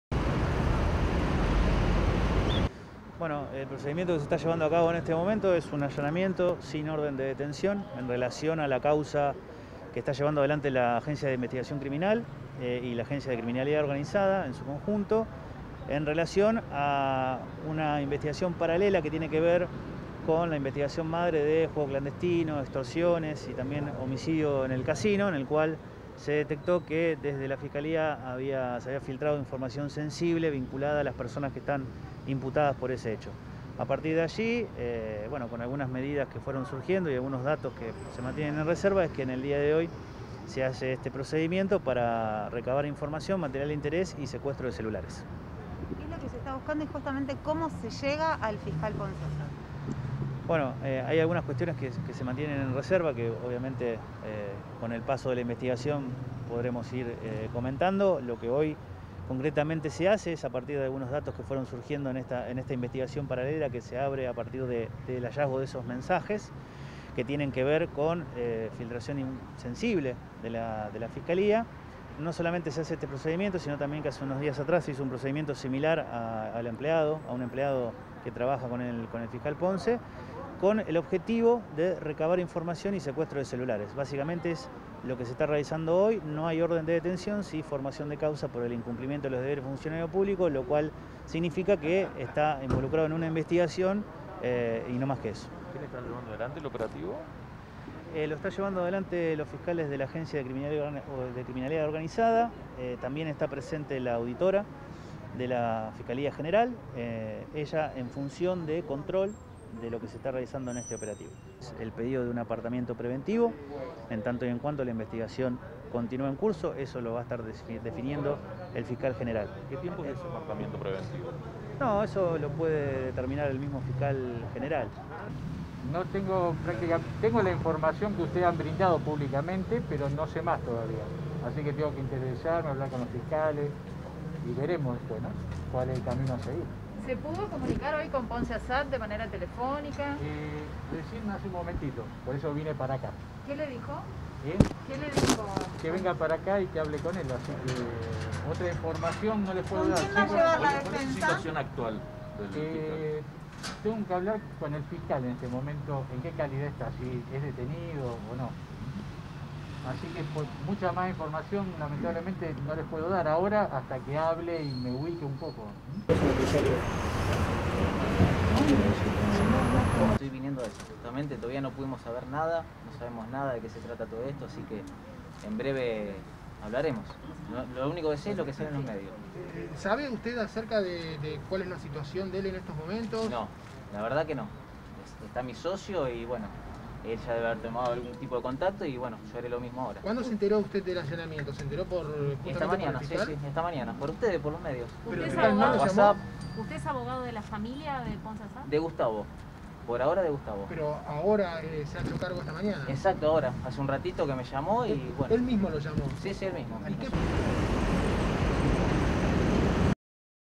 Caso Fiscal Ponce Asahad. Investigan filtrado de información de la Fiscalía.
INVESTIGAN CORRUPCIÓN LIGADA AL JUEGO CLANDESTINO

HABRÍA FILTRADO INFORMACIÓN DE FISCALÍA A DELINCUENTES

Demoran al fiscal Ponce Asahad por sospechas de corrupción ligadas al juego clandestino. Está sospechado de ser quien envió información reservada a una banda investigada por juego clandestino y extorsiones Este jueves había sido allanado un colaborador de él

Fue en la mañana de éste viernes que personal de la Tropa de Operaciones Especiales (TOE) realizó un allanamiento en BV Oroño 1217.

La causa de Casinos Clandestinos investigada en la Justicia ya contaba entre los detenidos con un ex Comisario de la Policía Provincial. En una de las audiencias imperativas, se expuso como prueba una serie de audios donde integrantes de esa organización delictiva se “jactaba” de tener contactos en Fiscalía y que averiguarían los movimientos de ciertas causas. A partir de ese entonces comenzó una pesquisa paralela para tratar de dar con el denominado “topo”, encargado de filtrar información a los delincuentes. Todos los elementos coincidían en una persona: Gustavo Ponce Asahad, que desde marzo conducía una unidad especial creada para intervenir en causas relacionadas con infracciones a las disposiciones vigentes en la cuarentena.  La detención de Ponce Asahad fue ordenada por sus colegas de Delitos Complejos LUIS SCHIAPPA PIETRA, GISELA PAOLICELLI Y MATÍAS EDERY.